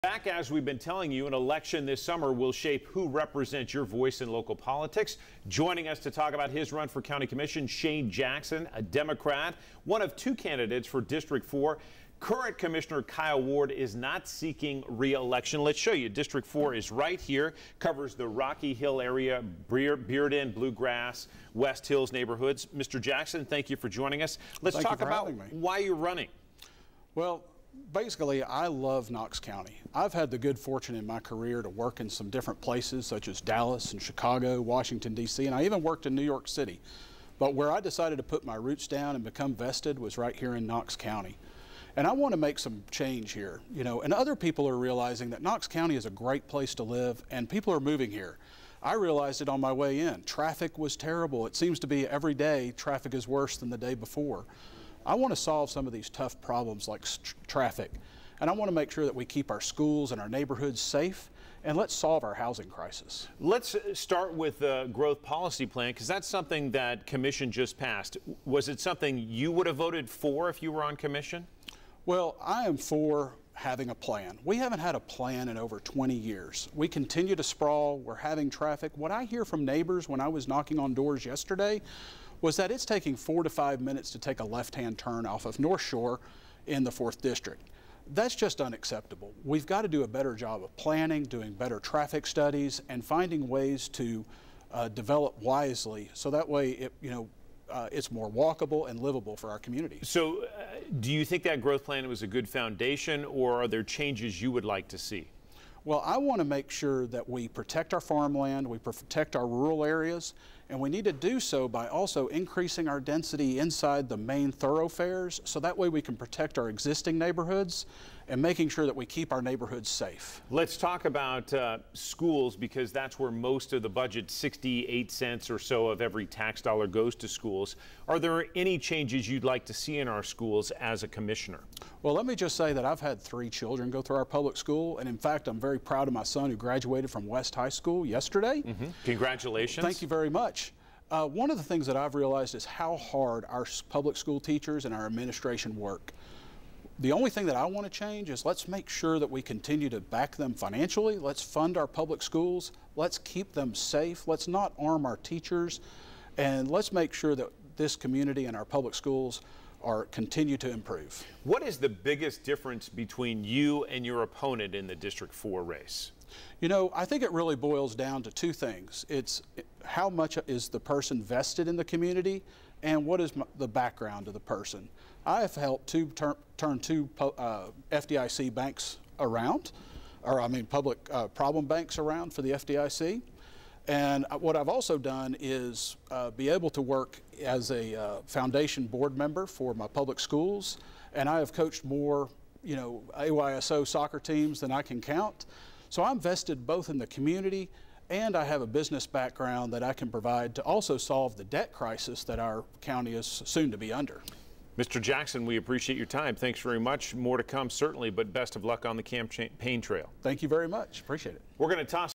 Before You Vote: Shane Jackson
Shane Jackson is one of two candidates running for the District 4 seat on the Knox County Commission.